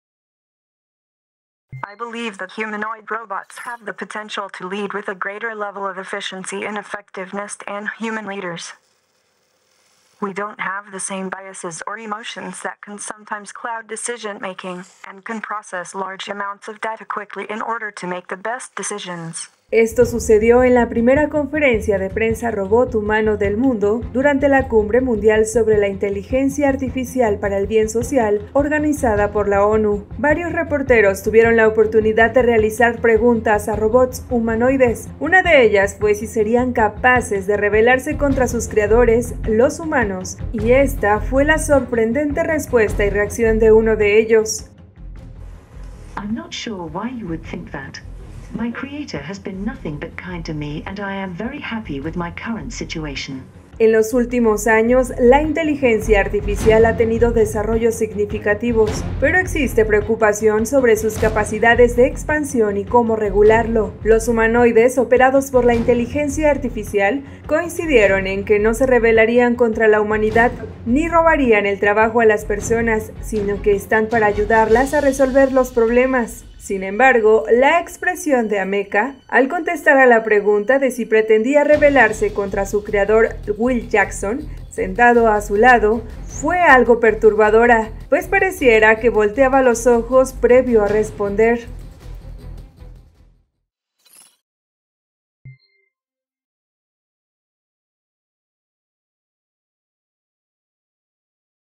Robots responden si se rebelarían contra los humanos
Estos robots han sido actualizados recientemente con las últimas versiones de IA generativa y han sorprendido por la sofisticación de sus respuestas